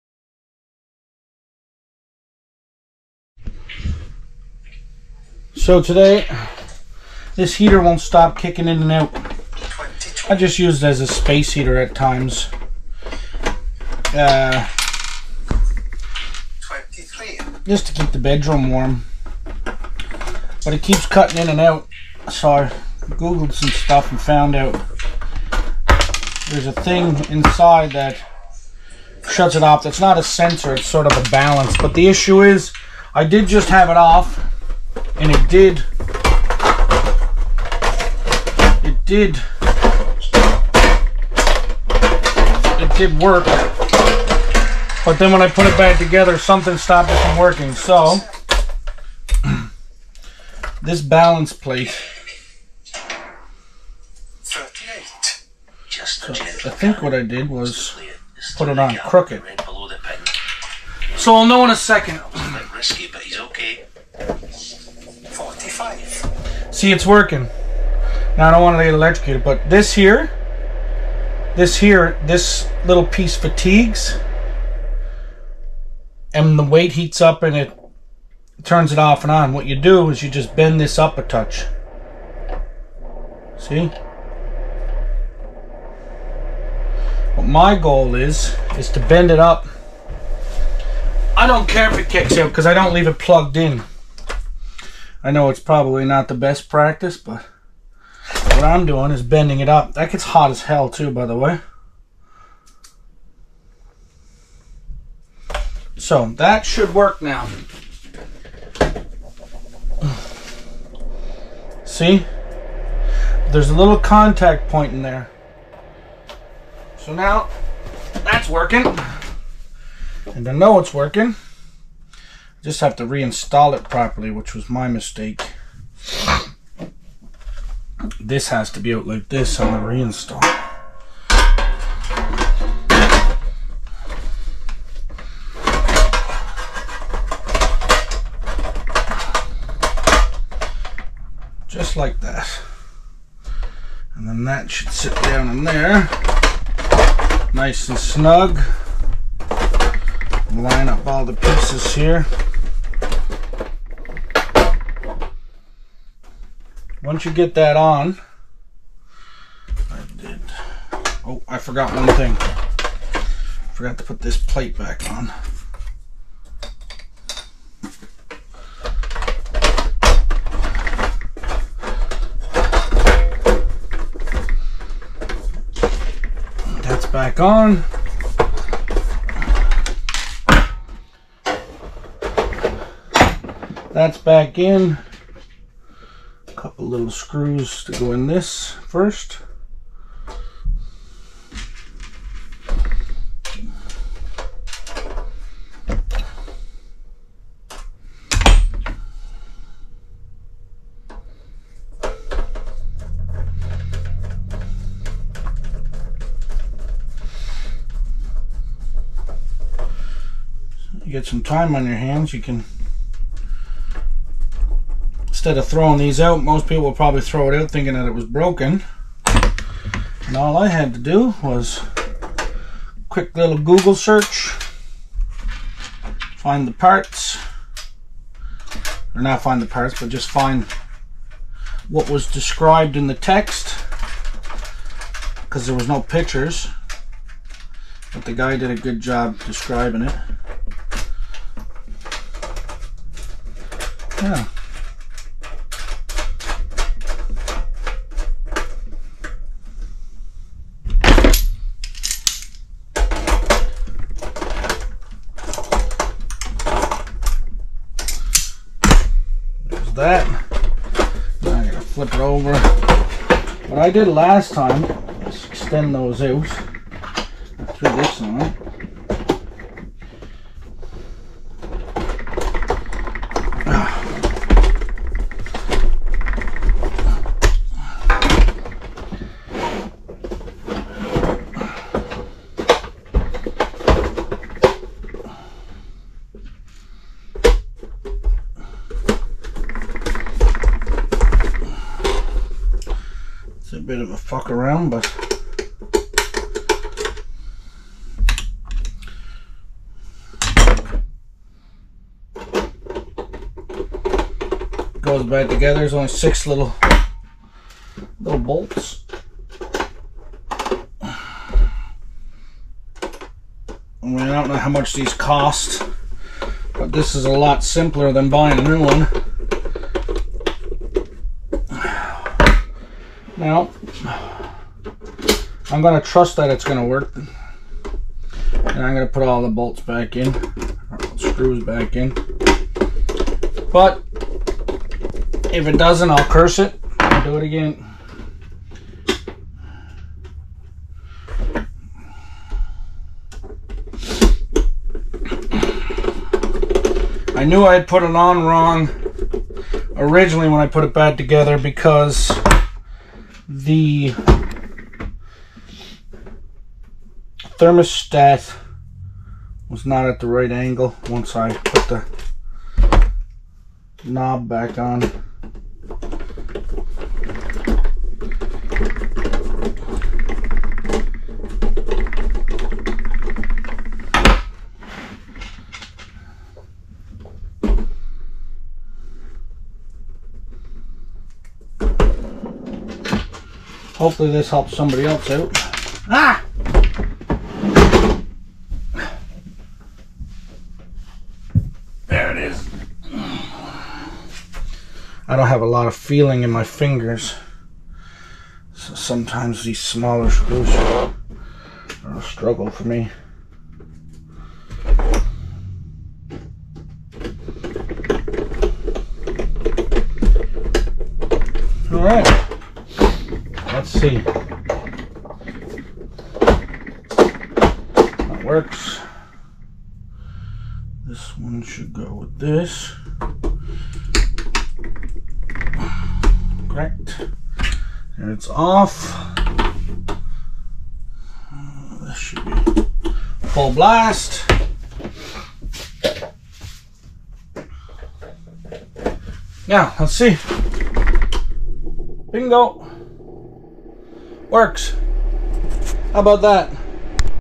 Honeywell HZ510 Fix
My HZ510 Honeywell heater kept cutting out every min or two and then starting again. After some short searches on google I found a fix but with no pictures of video so I made this one. It’s not perfect but it gives you more of a working view!